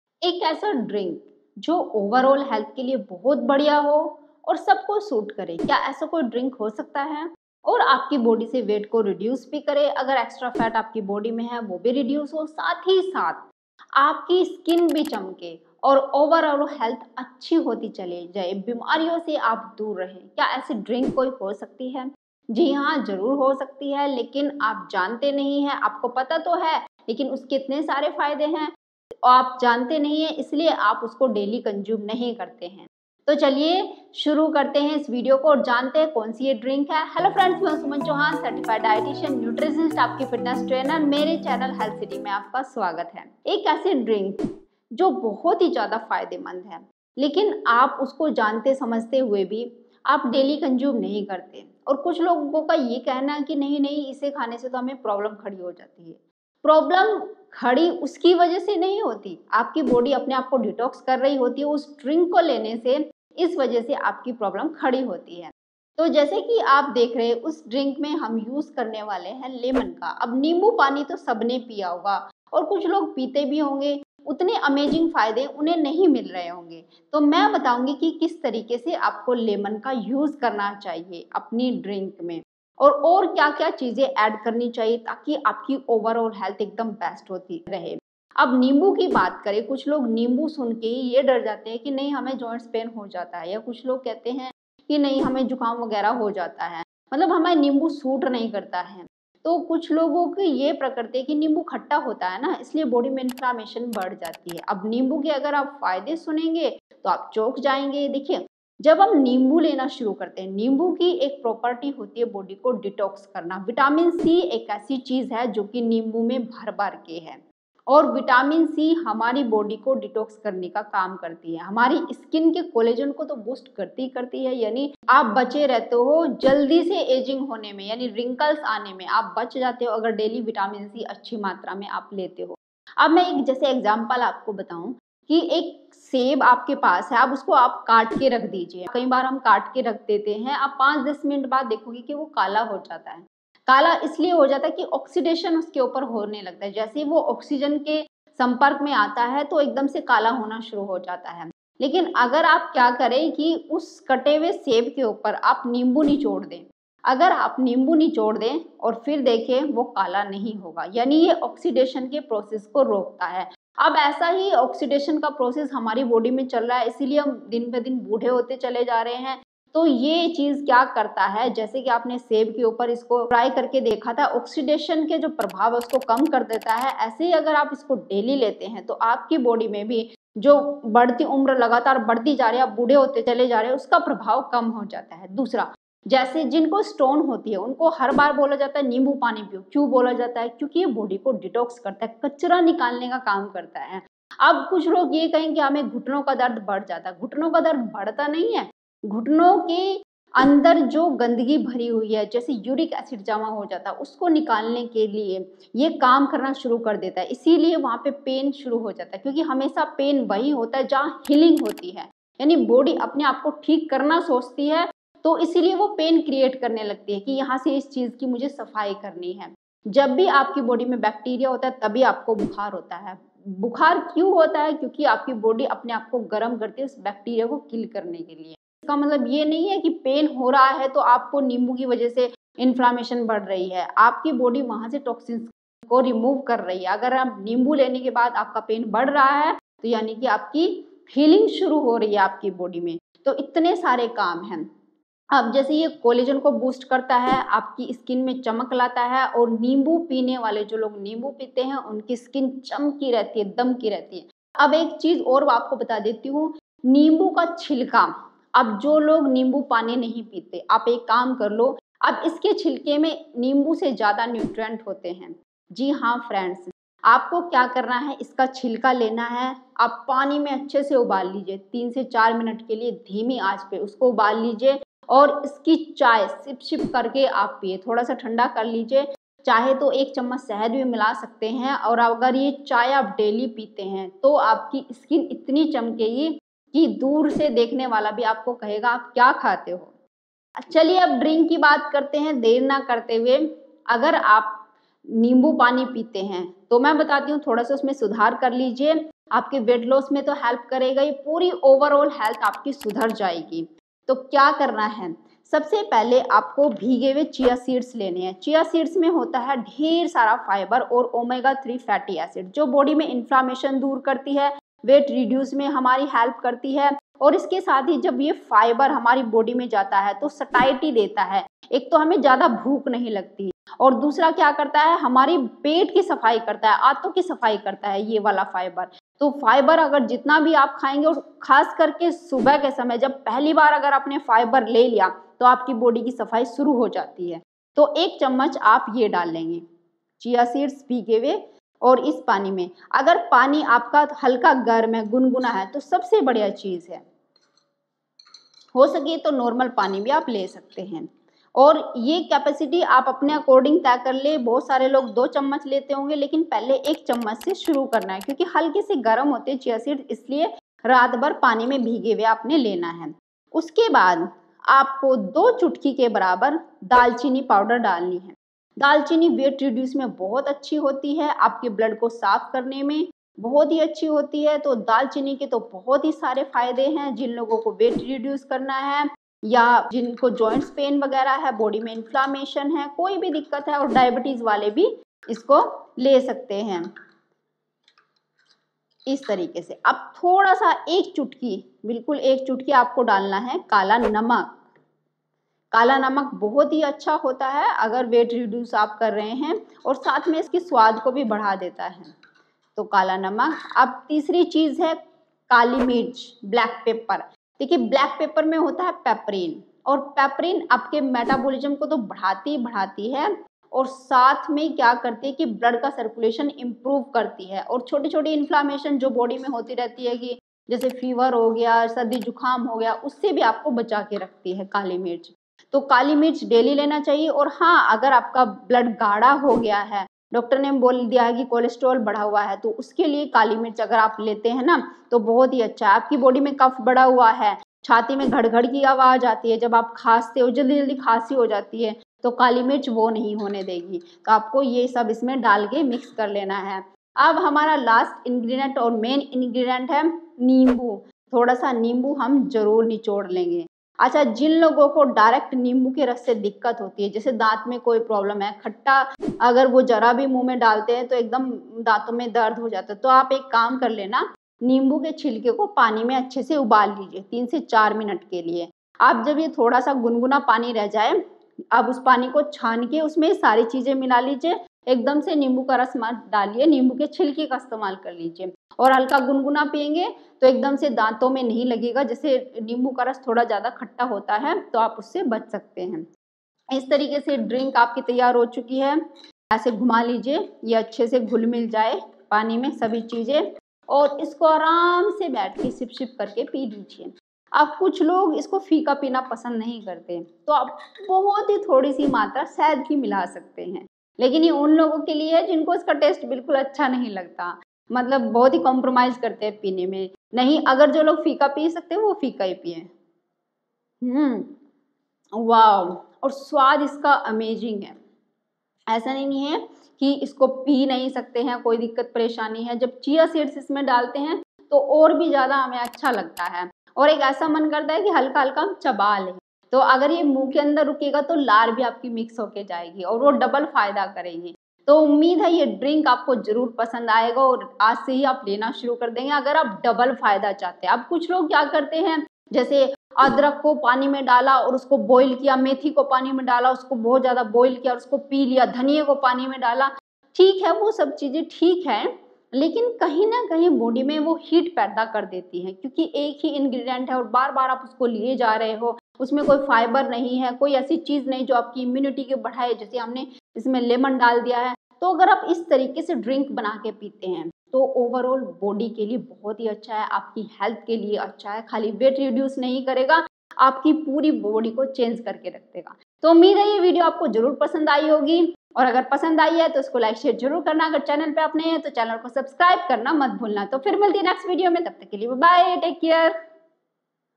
15 दिन में वेट और पेट की चर्बी ख़त्म, नो डाइट ,नो एक्सरसाइज़ | Weight loss Drink
15 दिन में वेट और पेट की चर्बी ख़त्म, नो डाइट ,नो एक्सरसाइज़ | Weight loss Drink